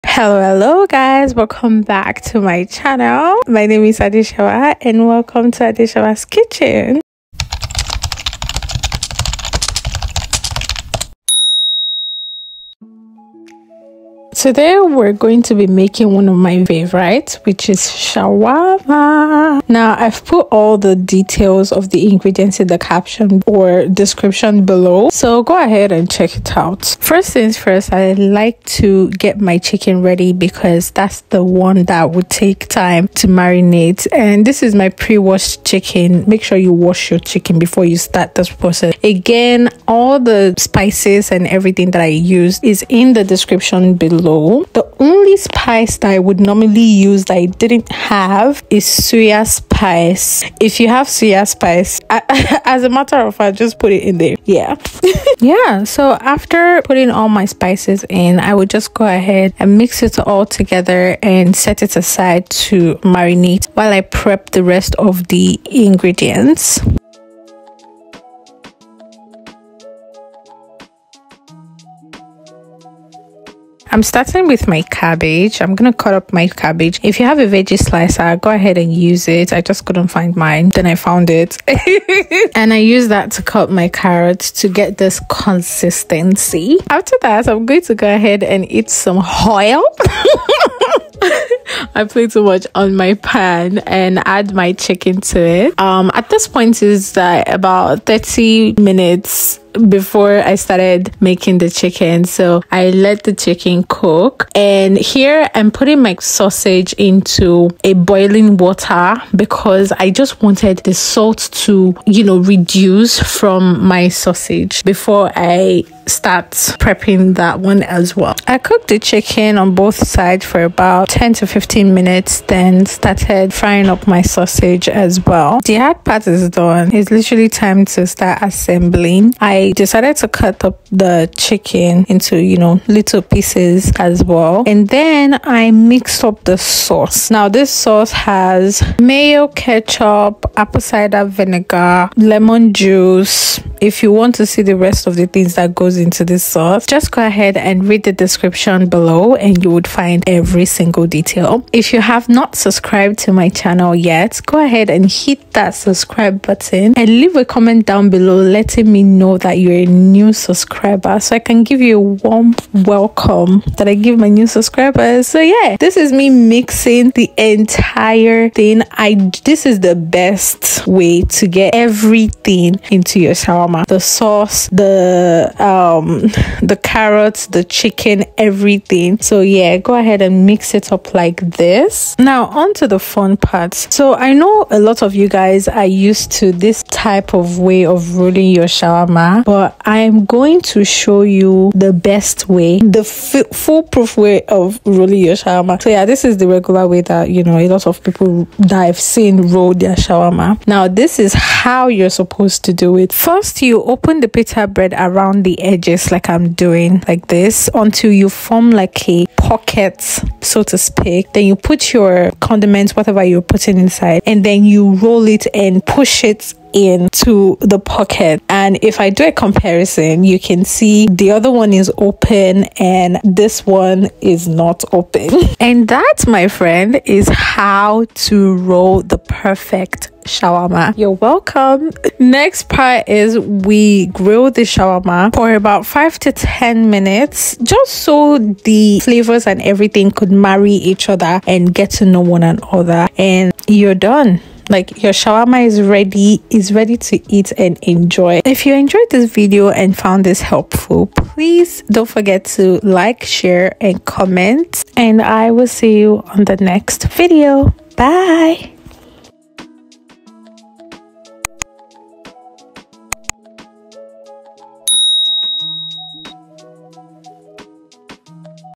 HOW TO MAKE SHAWARMA (Nigerian Style)
Nigerian Style Chicken Shawarma!

Ingredients:
- 2 Chicken Breasts
- 4 Beef Sausages
- 2 Carrots
- 1/2 Cabbage
- Pita Bread
- Seasoning Powder
- Salt
- Chilli pepper powder
- Sage
- Basil
- Lemon juice
- Paprika
- Curry Powder
- Garlic Powder

- Mayonnaise
- Ketchup
- 1/2 tbsp Lemon juice
- 1/2 tbsp Apple Cider Vinegar
- Chilli pepper
- 1 tsp Salt